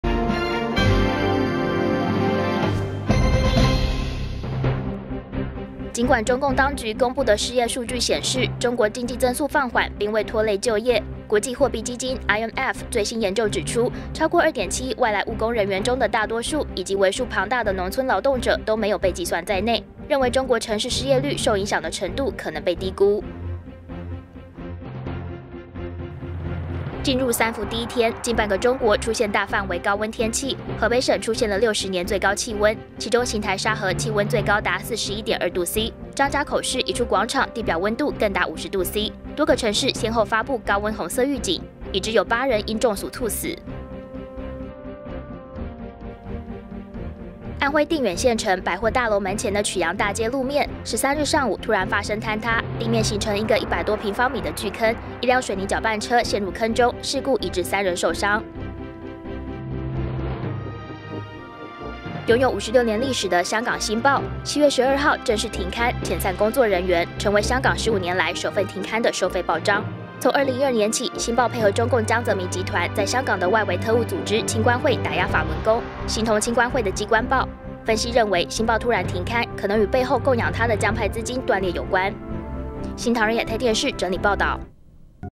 【新唐人/NTD】7月14日中國一分鐘 破60年最高極值 河北高溫8人熱死
IMF：中國就業市場壓力或被掩蓋

儘管中共當局公布的失業數據顯示，中國經濟增速放緩並未拖累就業，國際貨幣基金（IMF）最新研究指出，超過2.7億外來務工人員中的大多數，以及為數龐大的農村勞動者都沒有被計算在內。認為中國城市失業率受影響的程度可能被低估。

破60年最高極值 河北高溫8人熱死

進入三伏第一天，近半個中國出現大範圍高溫天氣。河北省出現了60年最高氣溫，其中邢台沙河氣溫最高達41.2℃，張家口市一處廣場地表溫度更達50℃。多個等城市先後發布高溫紅色預警。已知有8人因中署猝死。

安徽路面現巨坑 吞一水泥攪拌車

安徽定遠縣城 百貨大樓門前的曲陽大街路面13日上午突然發生坍塌，地面形成一個100多平方米的巨坑。一輛水泥攪拌車陷入坑中，事故已致3人受傷。

香港新報停刊 疑涉江派資金斷裂

擁有56年歷史的香港《新報》，7月12號正式停刊，遣散工作人員。成為香港15年來首份停刊的收費報章。從2012年起，《新報》配合中共江澤民集團在香港的外圍特務組織「青關會」打壓法輪功，形同「青關會」的「機關報」。分析認為，《新報》突然停刊，可能與背後供養它的江派資金斷裂有關。